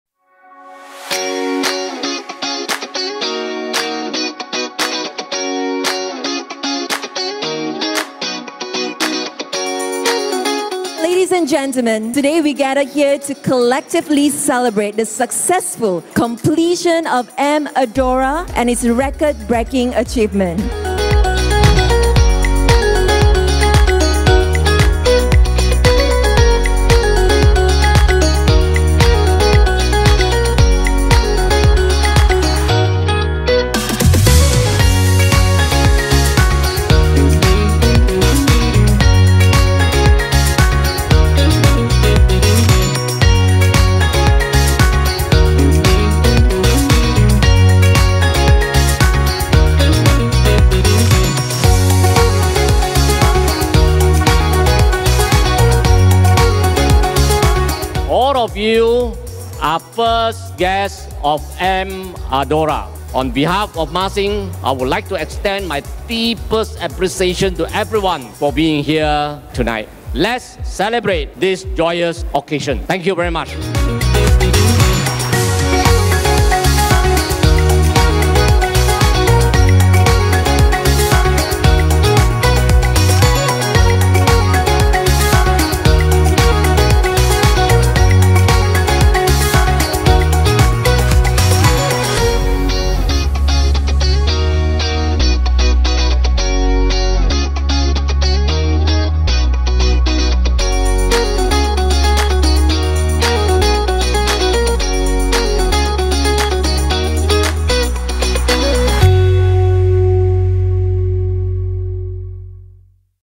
M Adora - Appreciation Night Event
Recently, Mah Sing Group hosted an exclusive Appreciation Night event at M Adora in Wangsa Melawati to commemorate the development's early completion and several notable achievements as well, with invitations extended to our valued partners, vendors, and contractors who significantly contributed to the completion of M Adora.

The accomplishments, which include obtaining the highest QLASSIC score of 85% for construction certification by the Construction Industry Development Board (CIDB) among Mah Sing’s M Series developments, and achieving 2.2 million man-hours without a Lost Time Injury (LTI) throughout the construction process, firmly highlight our unwavering commitment to delivering excellence in service, maintaining sustainability in operations, and consistently providing top-tier, affordable housing solutions.

We would like to take this opportunity to extend our sincerest appreciation for the dedicated work from all parties involved. With unwavering commitment, we will continue striving to create outstanding developments that play a transformative role in our nation's progress.